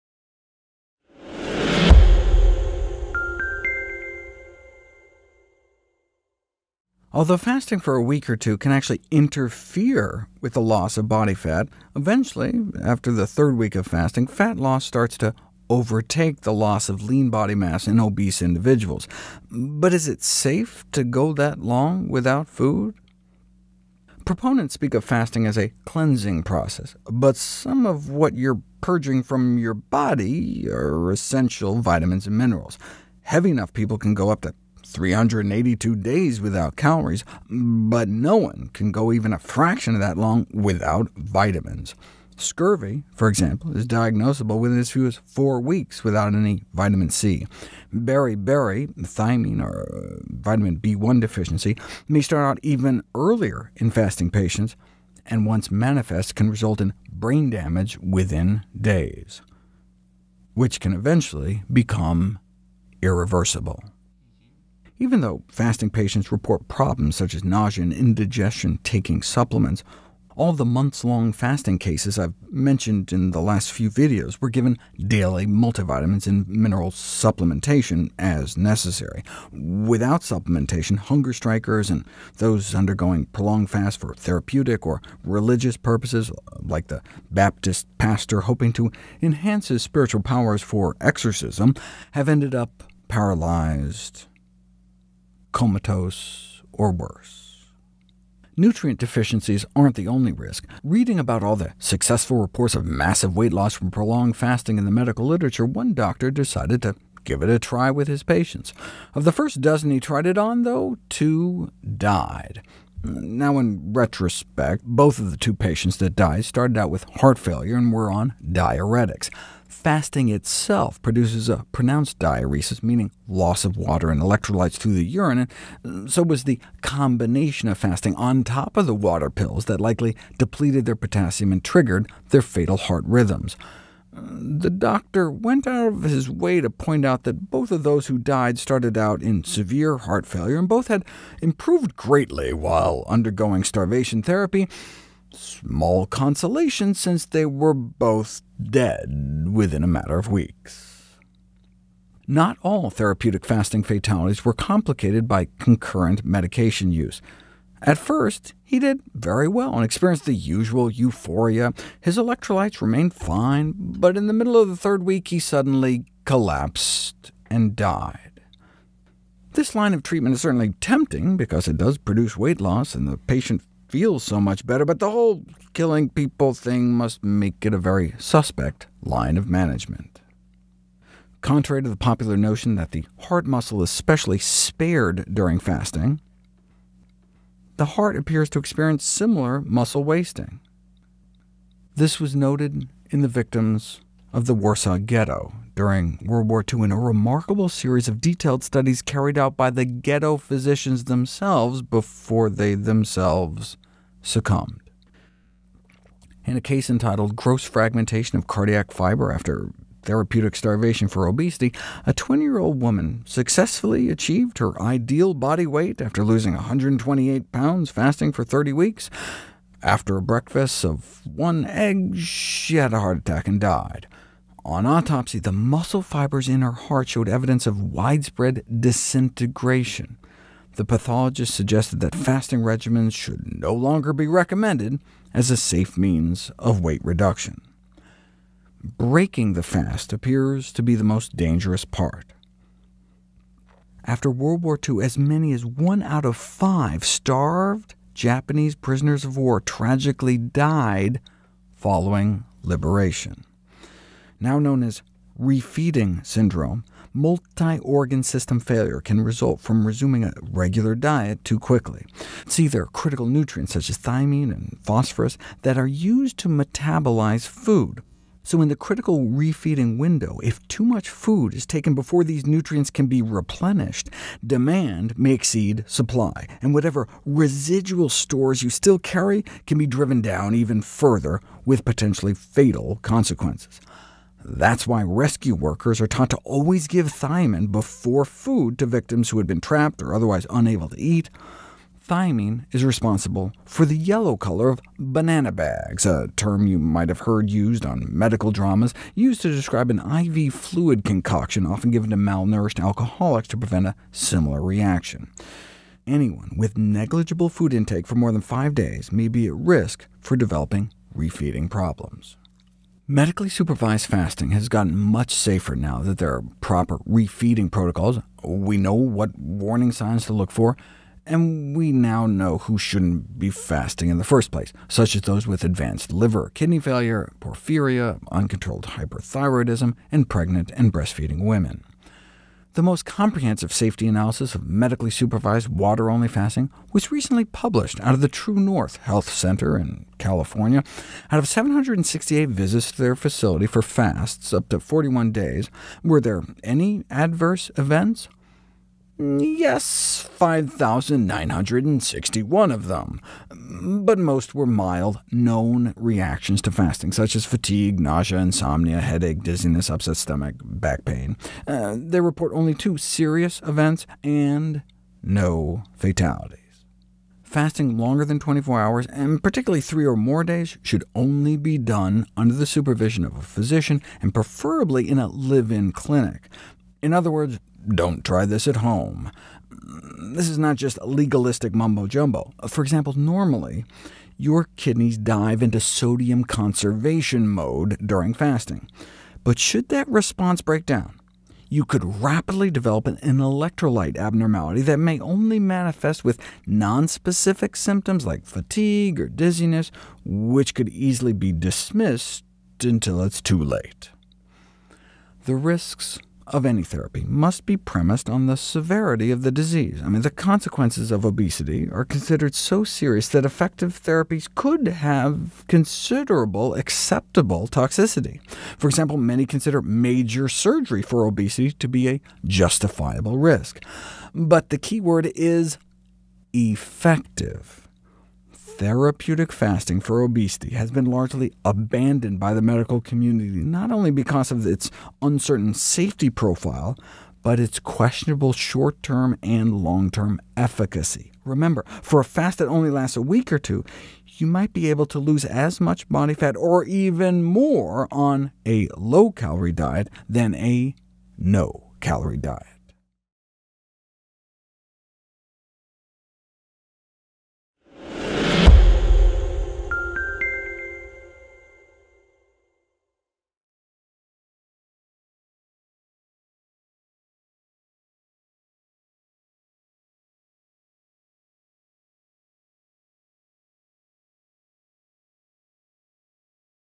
Fasting for Weight Loss Safe
Is Fasting for Weight Loss Safe?
The reasons why fasting longer than 24 hours, and particularly three or more days, should only be done under the supervision of a health professional and preferably in a live-in clinic.